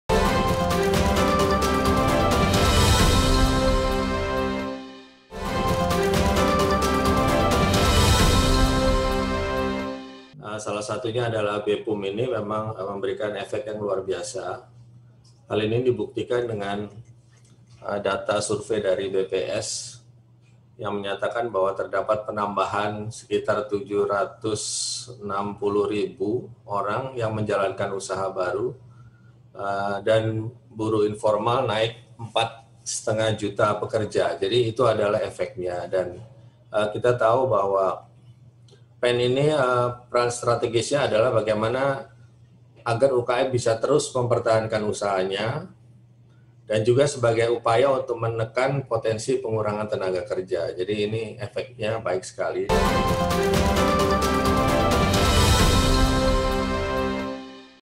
Pengusaha Baru Tumbuh 760 Ribu Orang, Buruh Informal Naik 4,5 Juta Pekerja
Asisten Deputi Koperasi dan UMKM Kementerian Perekonomian Iwan Faidi menyatakan bahwa  Bantuan Presiden Usaha Mikro (BPUM) berhasil memberikan dampak positif bagi pertumbuhan ekonomi.
"Banpres Usaha Mikro (BPUM) memberikan efek luar biasa. Hal ini dibuktikan dengan data survei BPS yang menyebutkan, terdapat penambahan sekitar 760 ribu orang yang menjalankan usaha baru. Buruh informal naik menjadi 4,5 juta pekerja. Jadi, itu adalah efek (positif)nya. Kita tahu bahwa Pemulihan Ekonomi Nasional (PEN) ini strateginya adalah, menemukan cara agar pengusaha terus mempertahankan usahanya, dan sebagai upaya untuk menekan potensi pengurangan tenaga kerja. Jadi ini efeknya baik sekali."